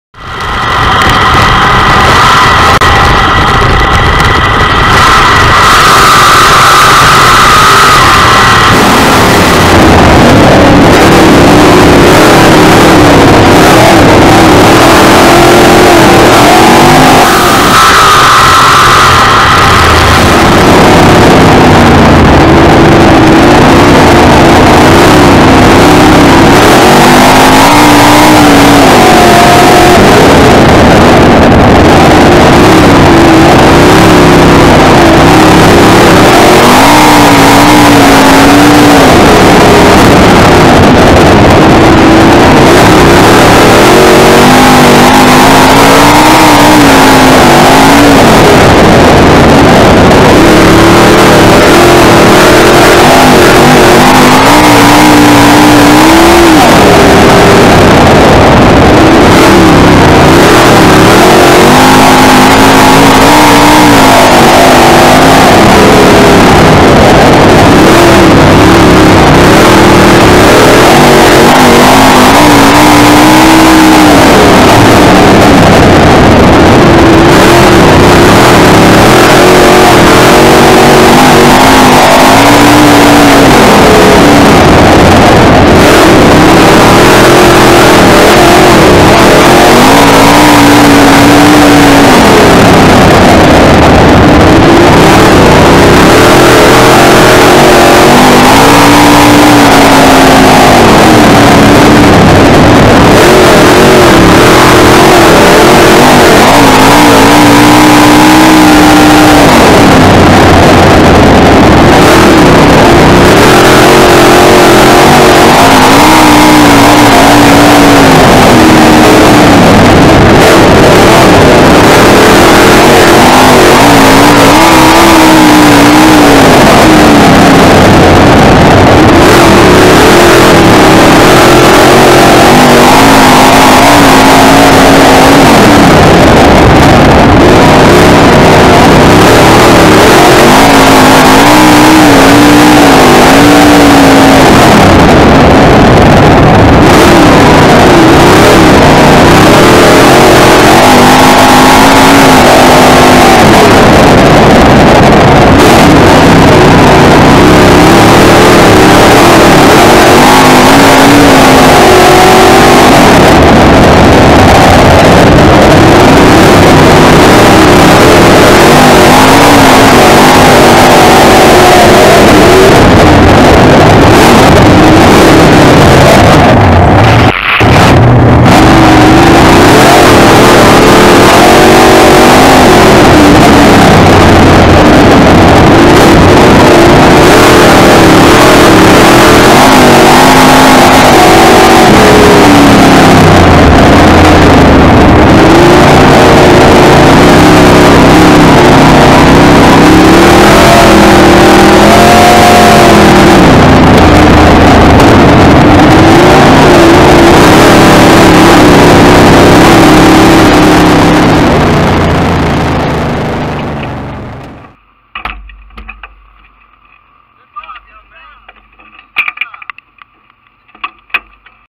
Collin Ambrose first Paragon sprint car heat win 7 2 2016
Collin Ambrose winning his first ever sprint car heat, in the rain no less, at Paragon, IN